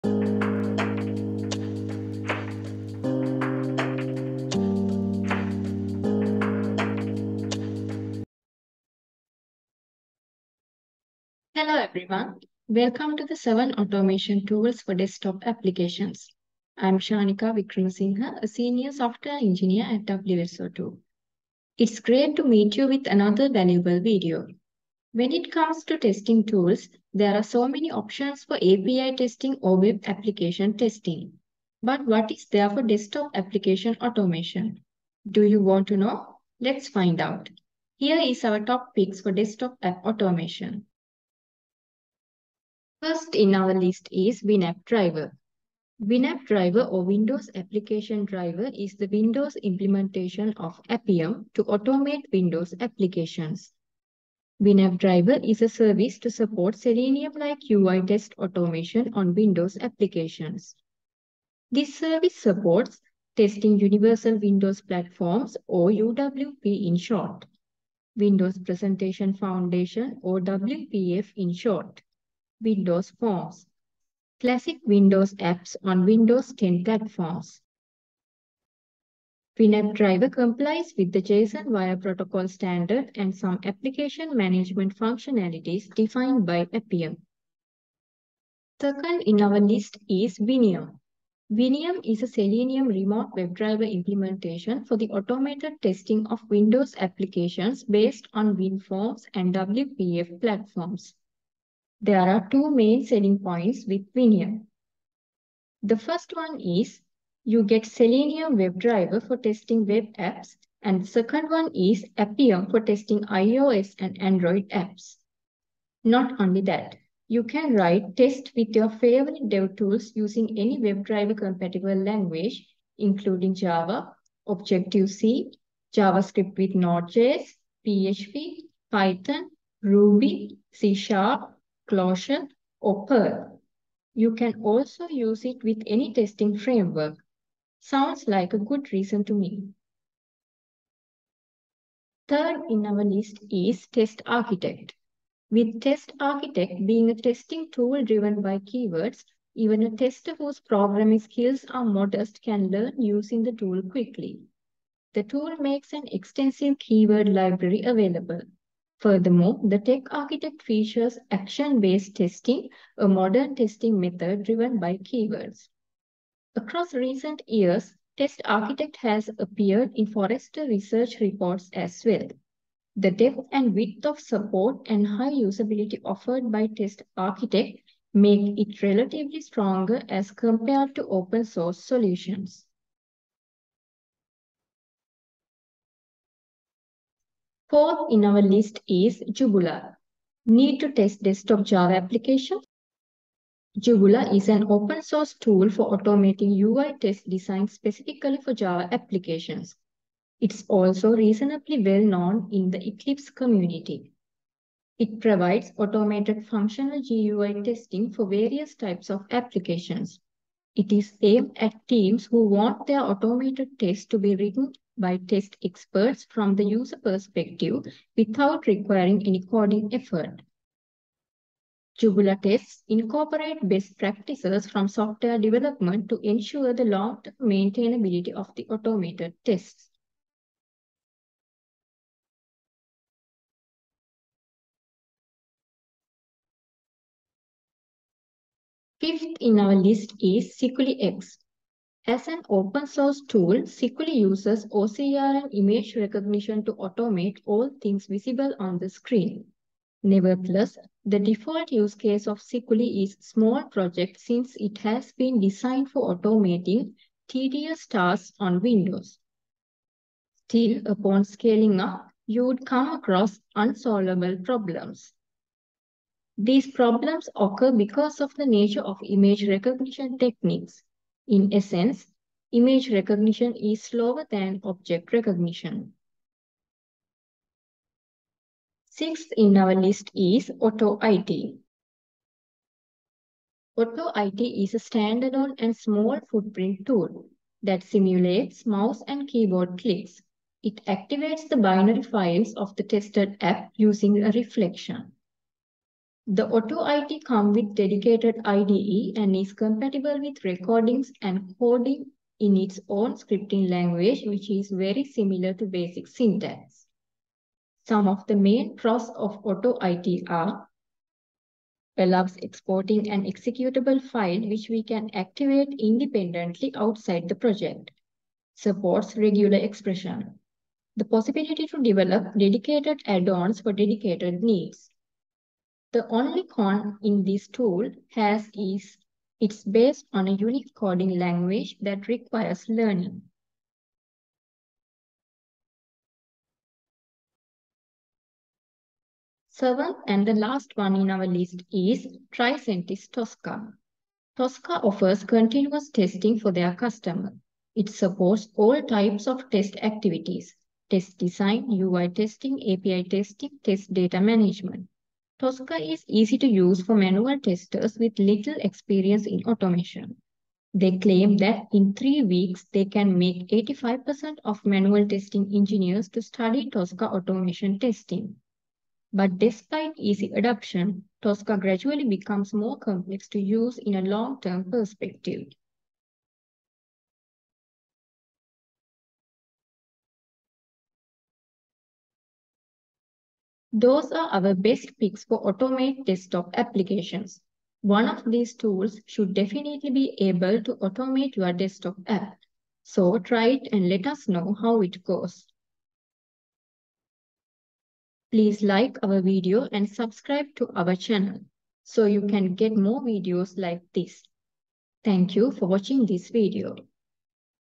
Top 7 Automation tools for Desktop apps
This video introduces to you the best automation tools available for automating Desktop apps. This video will be valuable for QA who are going to test desktop applications. In this video, we talk about the pros and cons of
WinAppDriver
Winium
TestArchitect
Jubula
Sikuli X
AutoIT
Tricentis Tosca

If you want to add any other tool to this video let us know. we will be covering how to use each of these tools in our future videos.